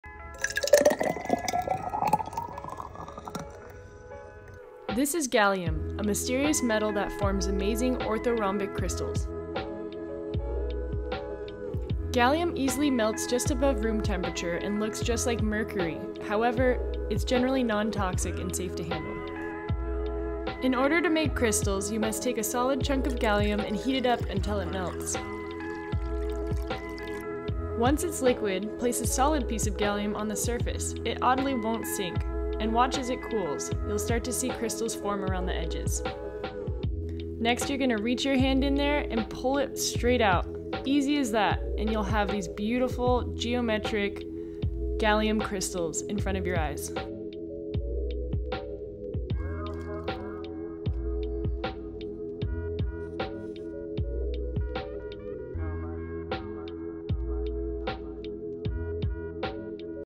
Making Gallium Crystals!✨
Gallium is absolutely incredible as it turns from solid to liquid to crystal form so easily. You can literally see the geometric crystals form in front of your eyes! This experiment really shows the process of crystal formation and how this material organizes itself into a precise geometry. Although these crystals form in a short amount of time and are temporary, crystals found in nature form over a long period of time and are more permanent which makes us appreciate them even more!💎❤️